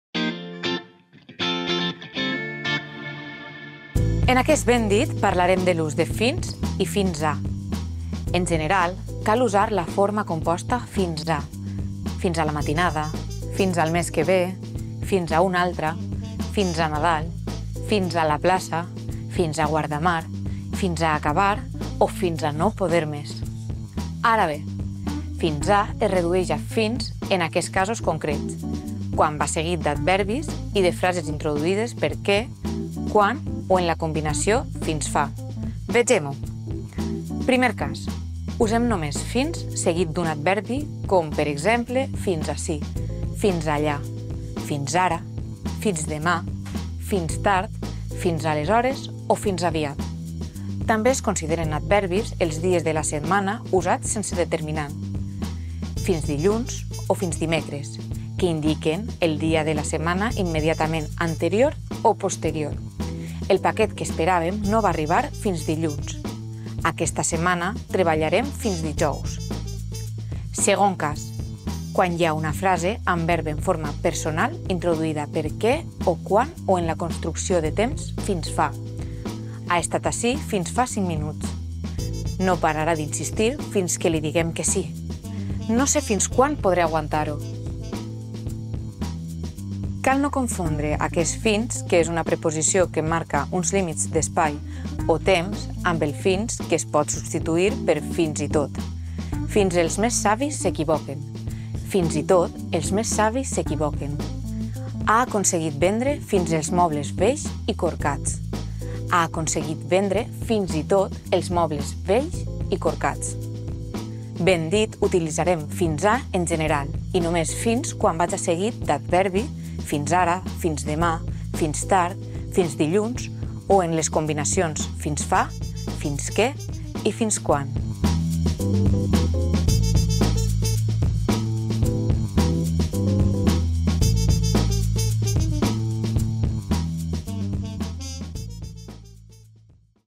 Ben dit! T2 | 48 Fins - fins a
T'expliquem com usar correctament les preposicions FINS i FINS A. Ara sí que ho diràs ben dit!